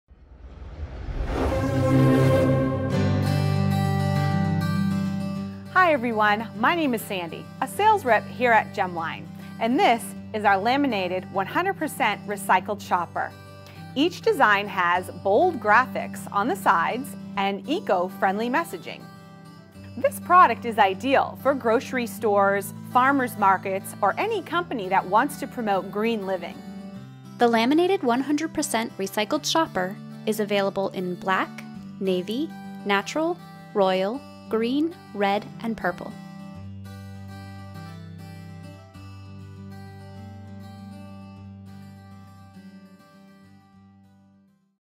Laminated 100% Recycled Shopper (#1697, #1698)
This fashionable and reusable tote is laminated to provide maximum durability and perfect for all your grocery essentials. The large main compartment has a generous gusset for maximum capacity.  It also has 23.5" shoulder straps. This product is made from 100% PET recycled fabric and three recycled water bottles and is kid-friendly/CPSIA compliant.